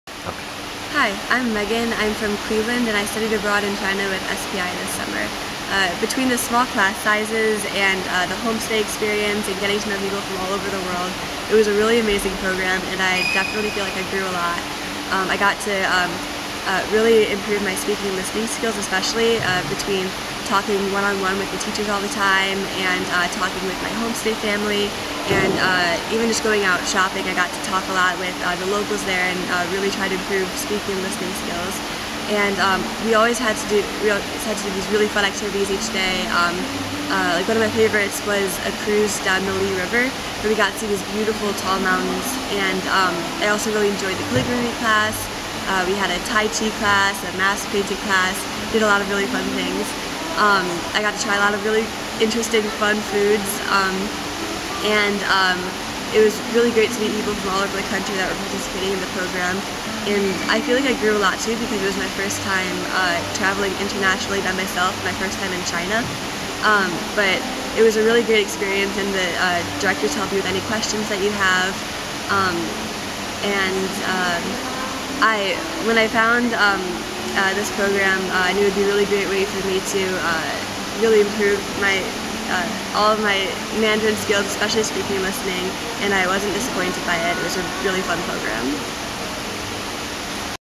Guilin, China - Megan - Solon High School - SPI High School Chinese Immersion Program
Watch 2017 SPI China alumni, Megan, describe her first experience with SPI Study Abroad's Mandarin Immersion in China.